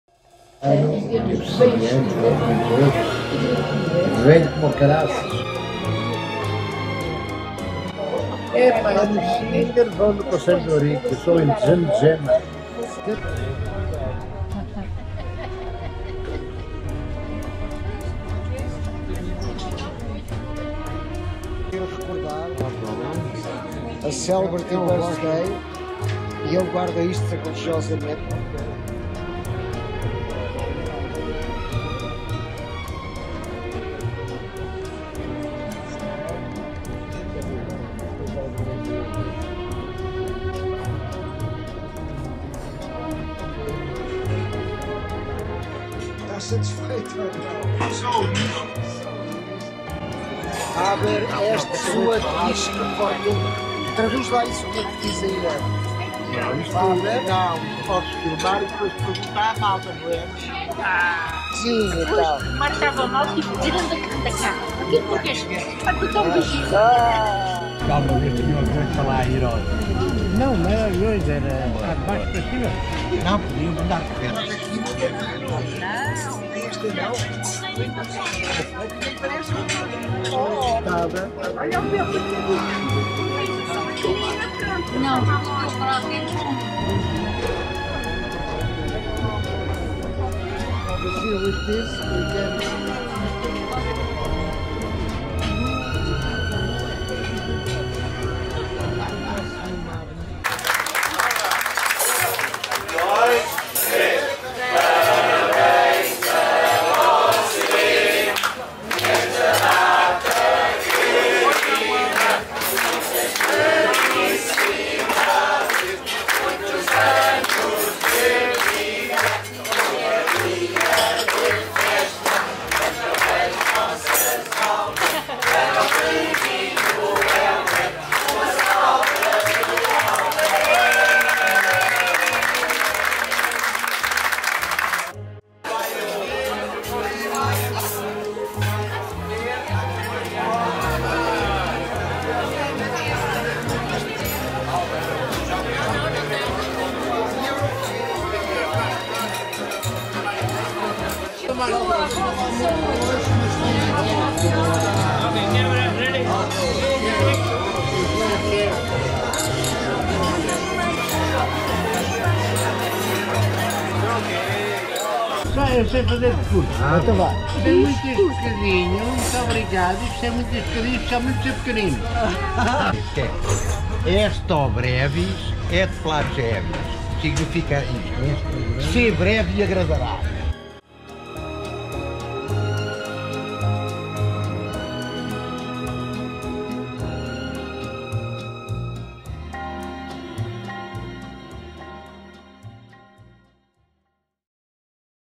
03.04.2019 - Aniversário do Hélder, no Costa Pacífica
No cruzeiro de Março /Abril pelas Caraíbas, e já no regresso à Europa, um grupo de amigos portugueses comemora o aniversário de um dos companheiros de viagem.